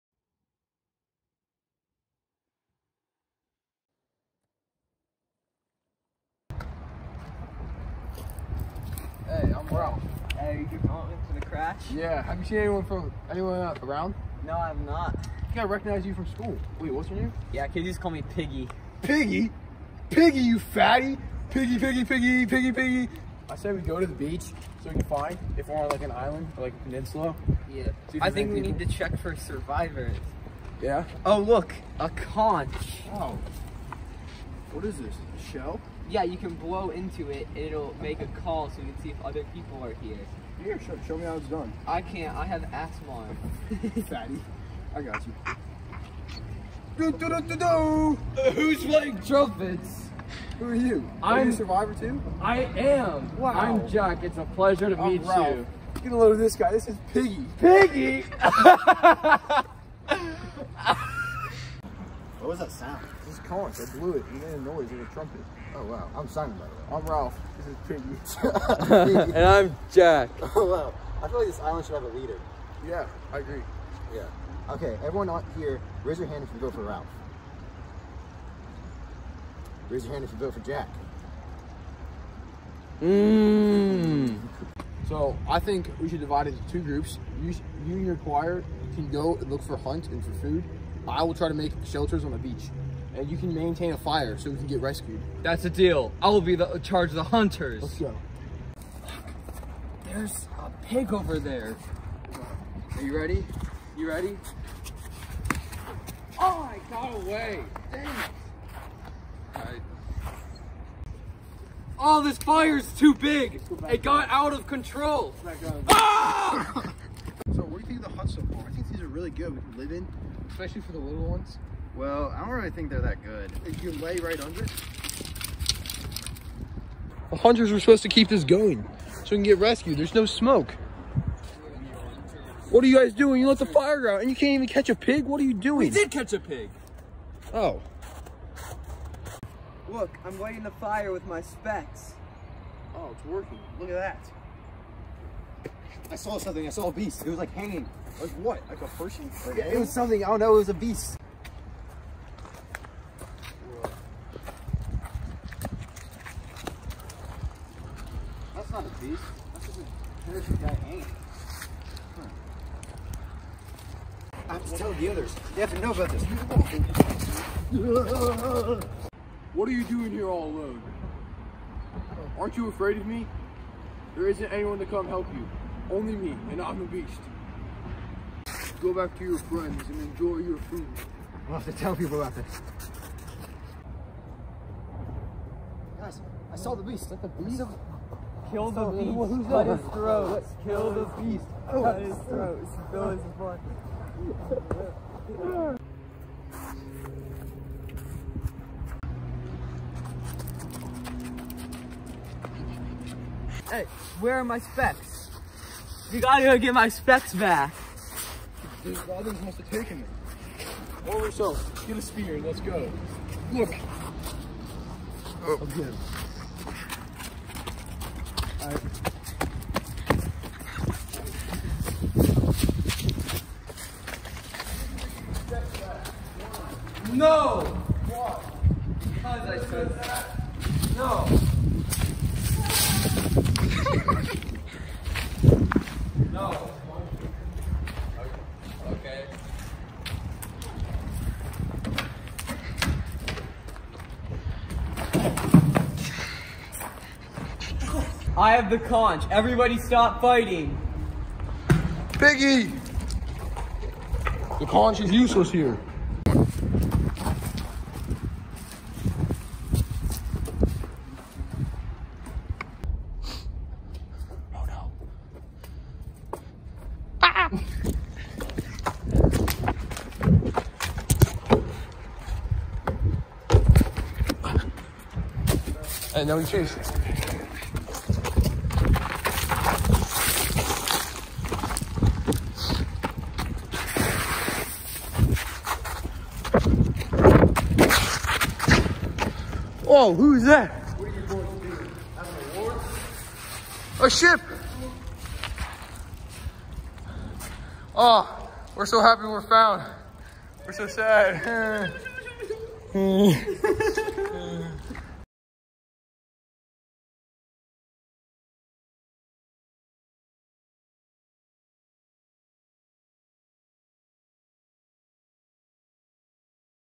THE LORD OF THE FLIES: Official Trailer (2022) | English 10
English trailer project on the lord of the flies.